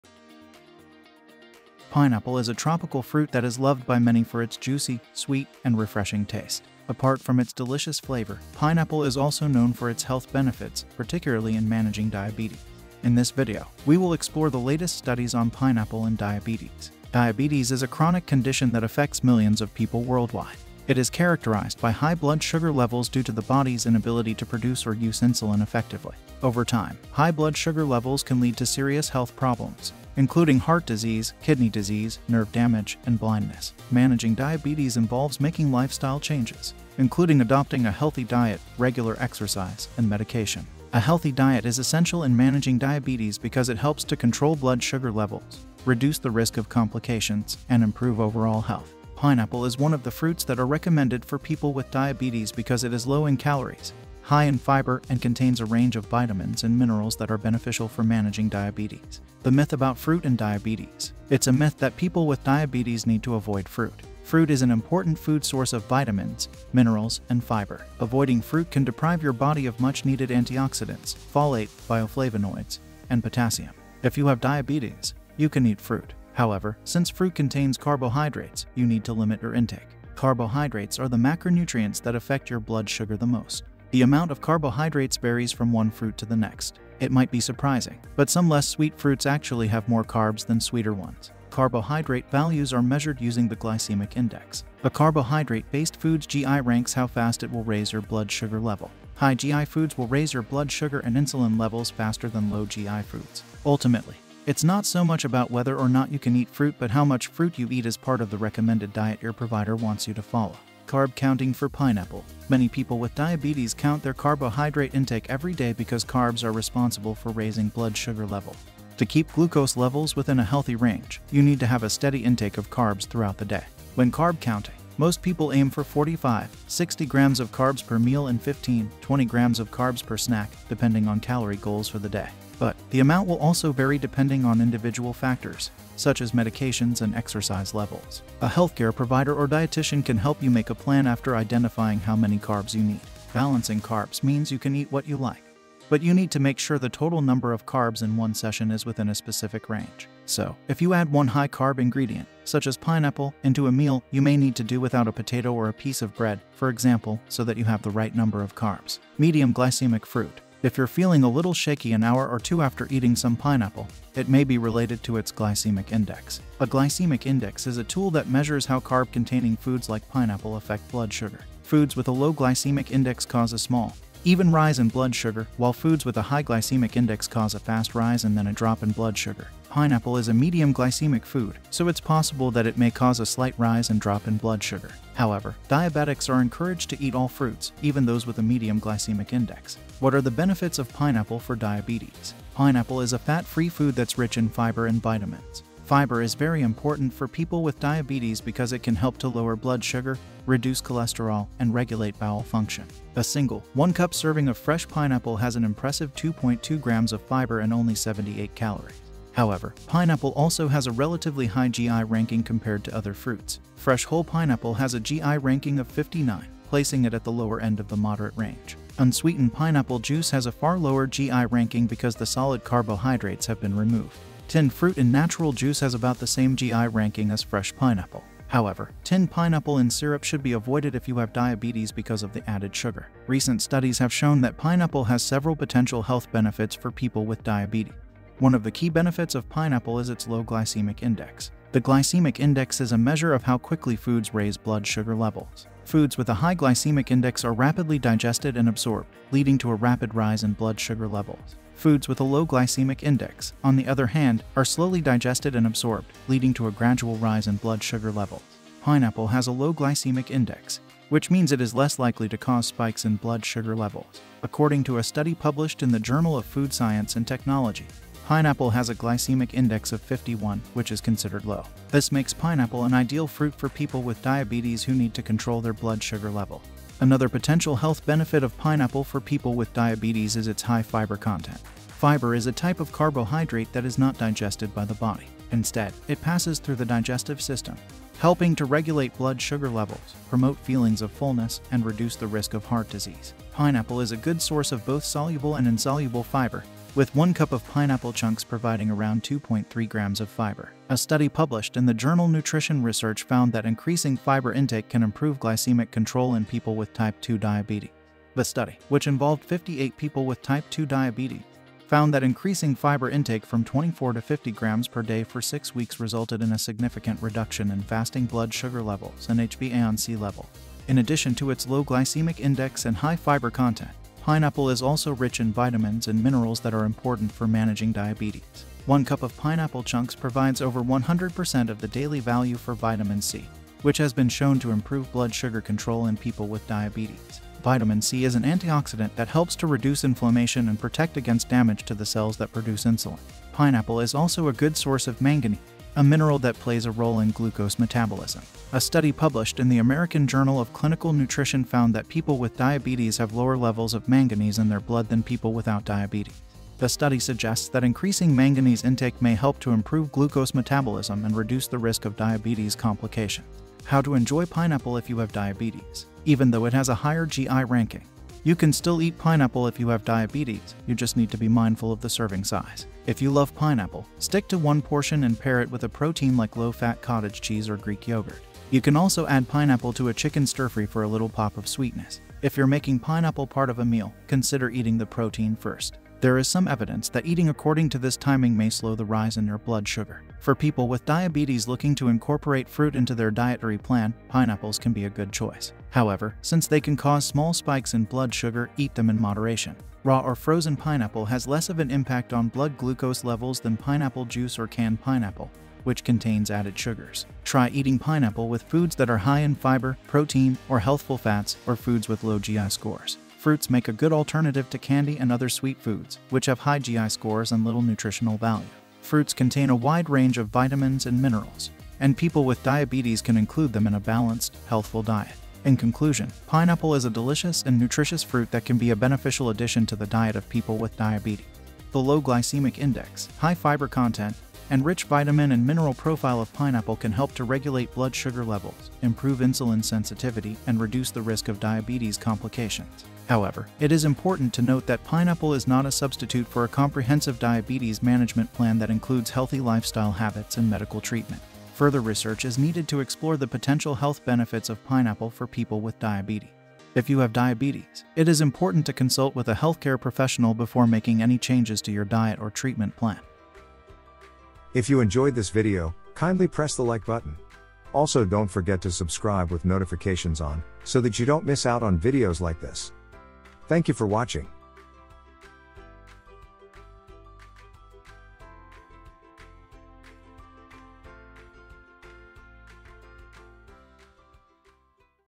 Why Pineapple is the New Superfood for People with Diabetes - Experts Weigh In
Pineapple is a tropical fruit that is loved by many for its juicy, sweet, and refreshing taste. Apart from its delicious flavor, pineapple is also known for its health benefits, particularly in managing diabetes. In this video, we will explore the latest studies on pineapple and diabetes.

Diabetes is a chronic condition that affects millions of people worldwide. It is characterized by high blood sugar levels due to the body’s inability to produce or use insulin effectively. Over time, high blood sugar levels can lead to serious health problems, including heart disease, kidney disease, nerve damage, and blindness.